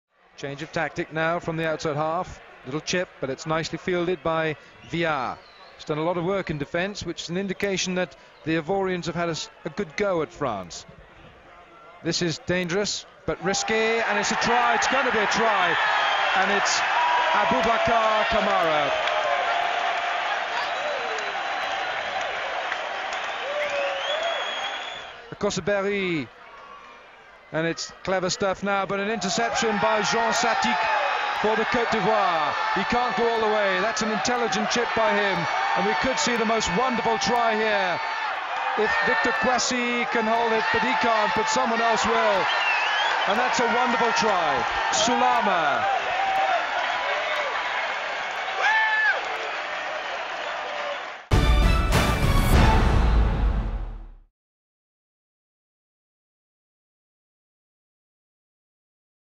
The Ivory Coast's First Ever RWC Tries!
Ahead of The Ivory Coast's African Cup Division 1B fixture against Tunisia later today, we look back at their first ever Rugby World Cup tries, scored twenty one years ago in RWC 1995 against France!